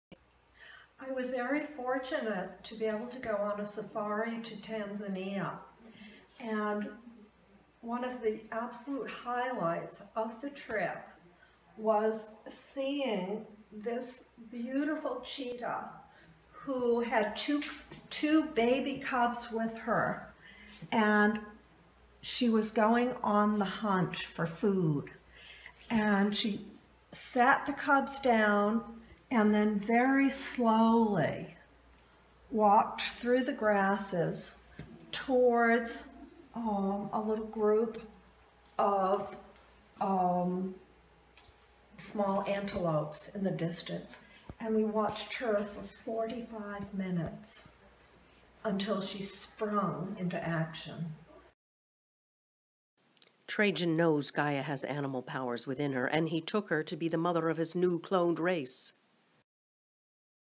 Saving Gaia artist Leslie Kramer observed endangered Cheetah on safari.
Kramer included grasses from Cape Cod in her monotype of the cheetah in Tanzania.